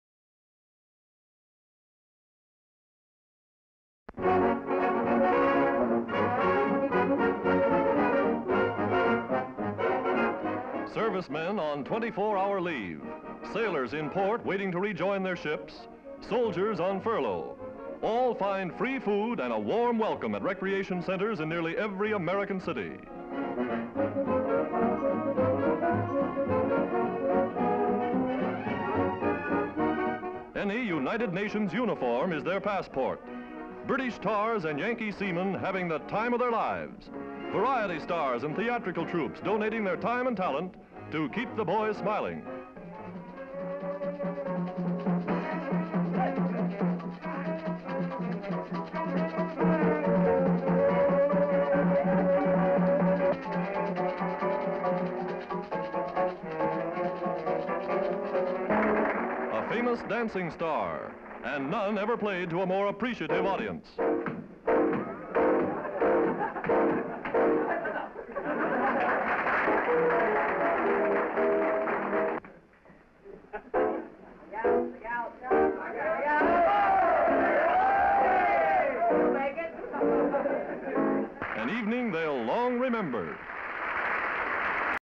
US soldiers eat food at recreation center and watch performances of stage stars i...HD Stock Footage
US soldiers eat food at recreation center and watch performances of stage stars in the United States.

Stage stars entertain the soldiers in United States during World War II. US sailors and soldiers look at a sign 'Stage Door Canteen'. Sailors at a port as they wait to rejoin their ship. Soldiers eat food received at a recreation center. Two sailors talk with a woman. A group of stage stars dance. Soldiers watch the dance. A stage star performs. Soldiers laugh and applaud. Location: United States. Date:  1942.

57,000+ broadcast-quality historic clips for immediate download.